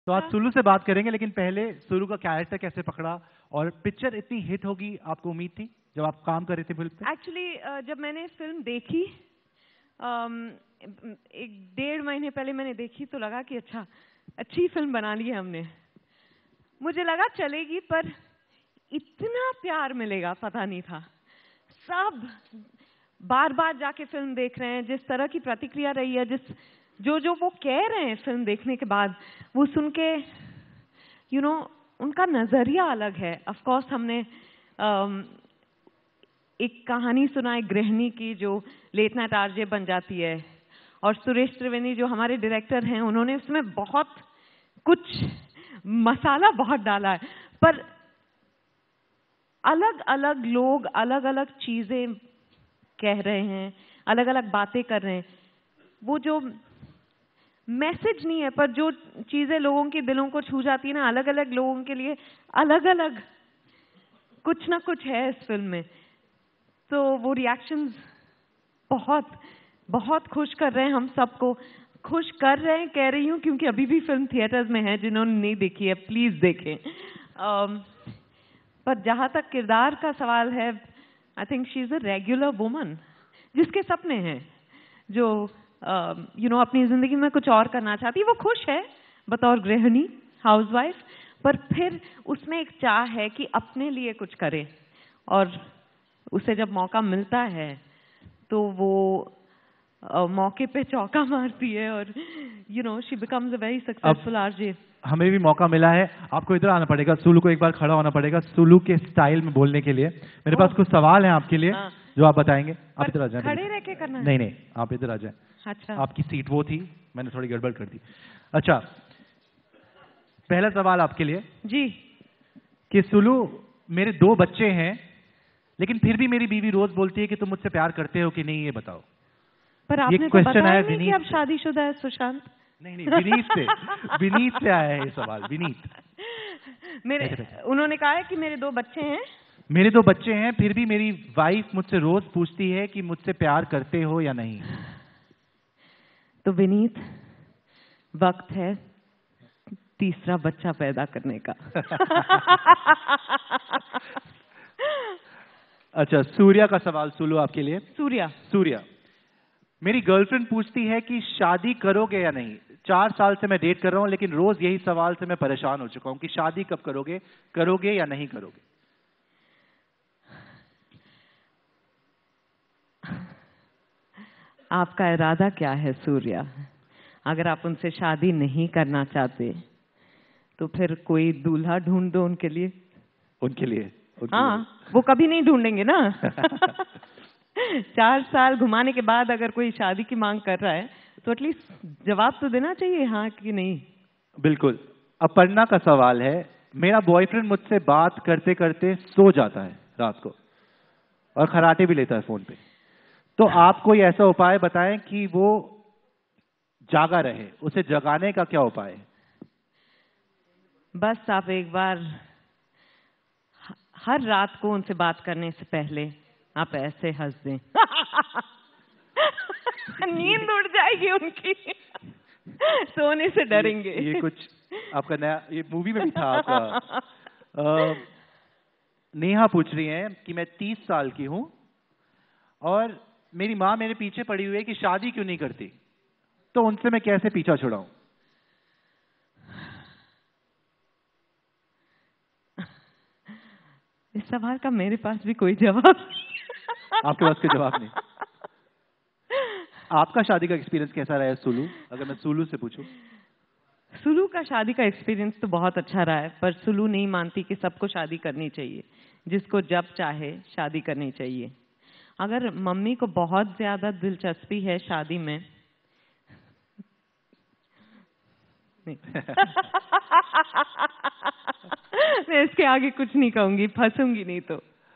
एजेंडा-आजतक 2017 | विद्या बालन ने जब 'सुलू' बनकर सवालों के जवाब दिय़े
एजेंडा आज तक 2017 के 'हमारी विद्या' सेशन में अभि‍नेत्री विद्या बालन पहुंची. उन्होंने यहां अपनी हालिया रिलीज फिल्म तुम्हारी सुलु के बारे में बताया. एक तरफ जहां विद्या ने फिल्म से अपने डायलॉग को सुनाया वहीं दर्शकों की भीड़ में भी कई लोगों ने उसी डायलॉग को दोहराया. वहीं, बॉडी वेट पर विद्या ने कहा, लोग आजकल काफी बॉडी ओबसेस्ड हैं. कोई मुझे मोटी बुलाए तो मुझे बुरा नहीं लगेगा, लेकिन कोई मेरे पर कमेंट करें तो मुझे अच्छा नहीं लगेगा.